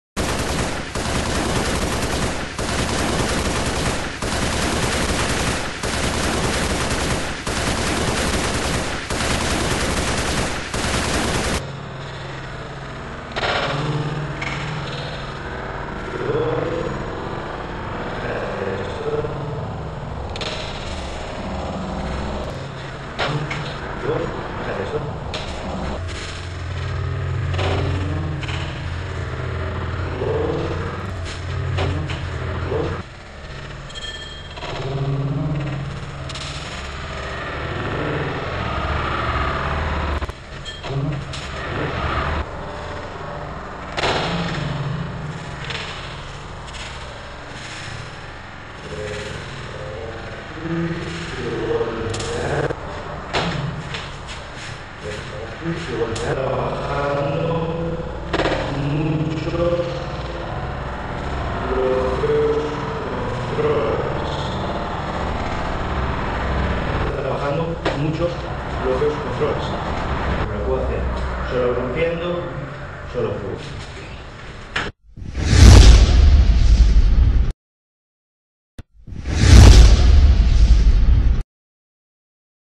KAHUNA Limalama
Un video de un momento del entrenamiento con el Maestro Iván Calva, representante de kahuna Internacional, en México, entrenando en la escuela sede da Kahuna México.